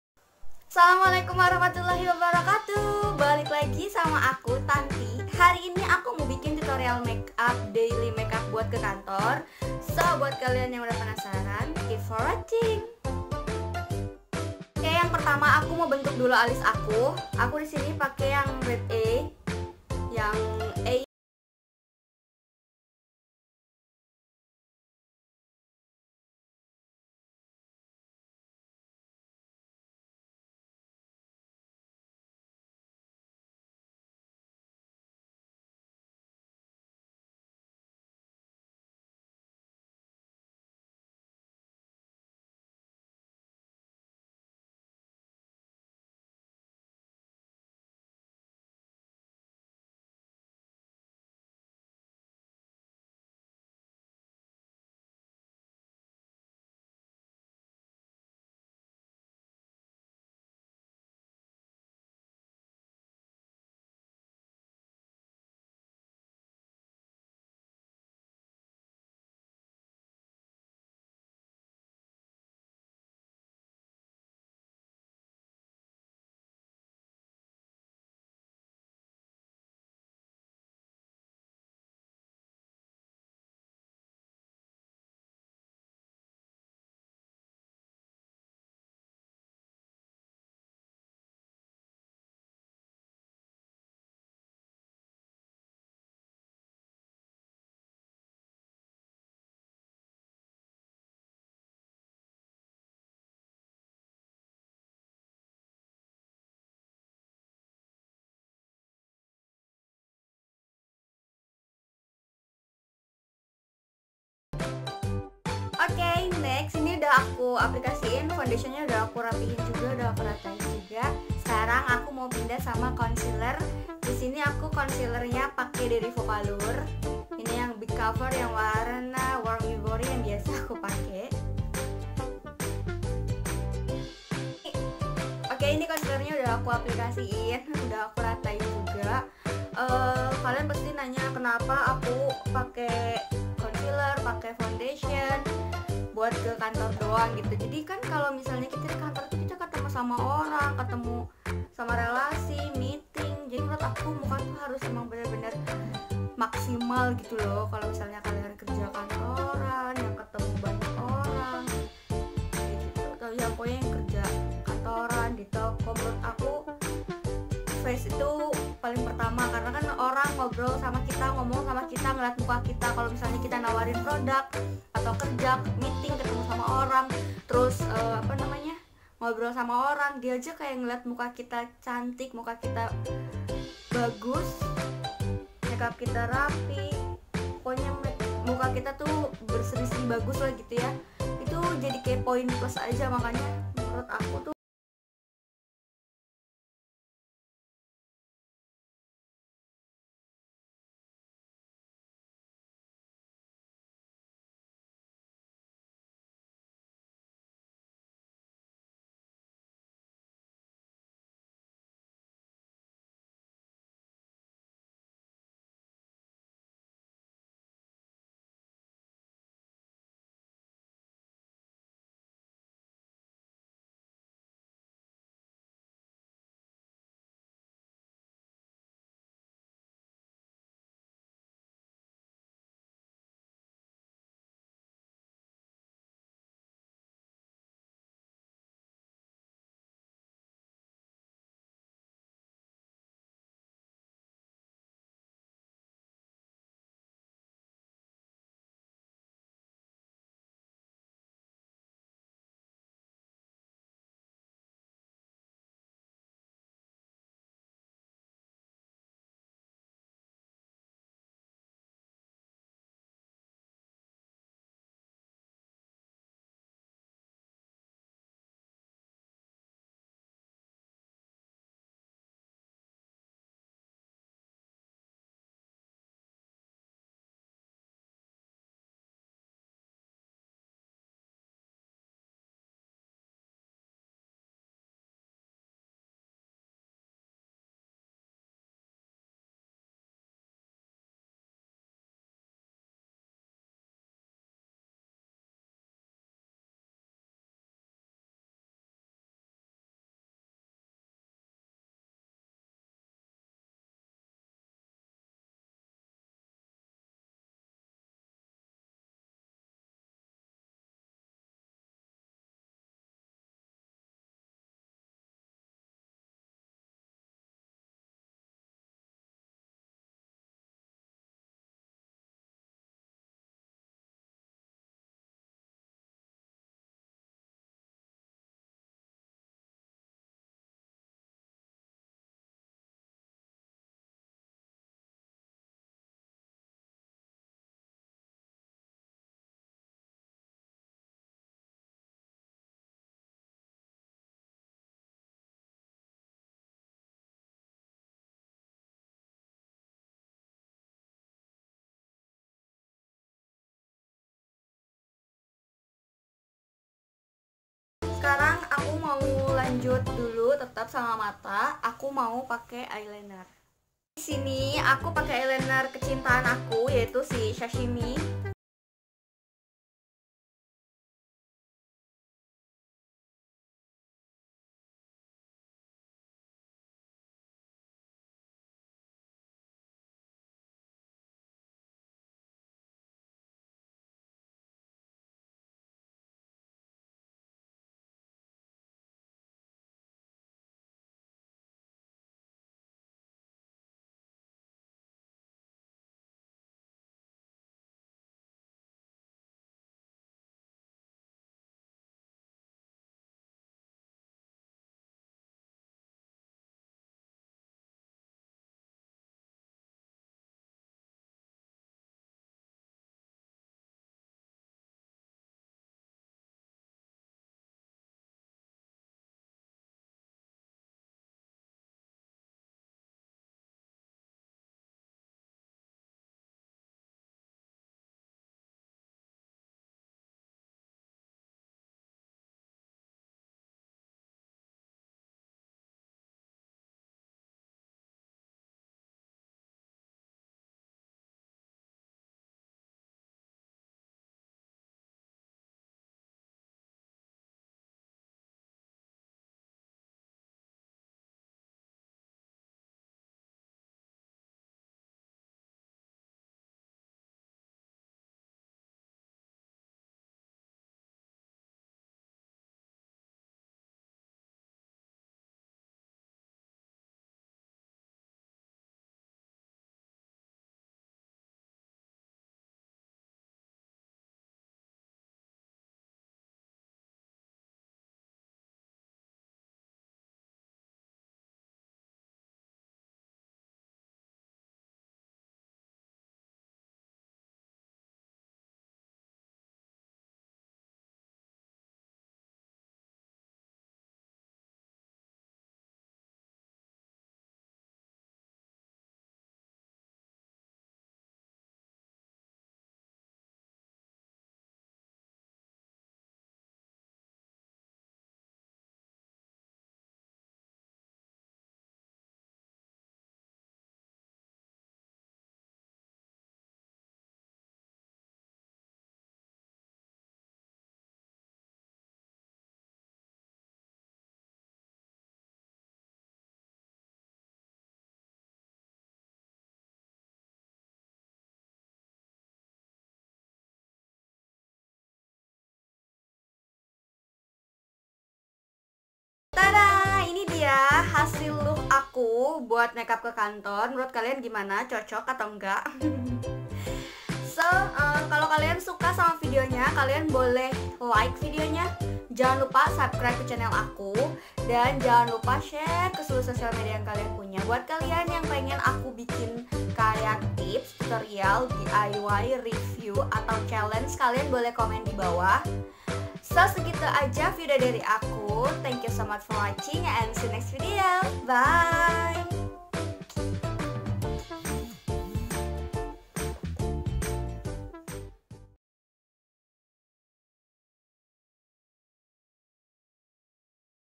TUTORIAL MAKEUP Daily Look Buat Kerja
Assalammualaikum
Welcome back to mu channel

Kali ini aku mau bikin Daily Look buat ke kantor atau kerja gitu.
So buat kalian yang udah penasaran keep for watching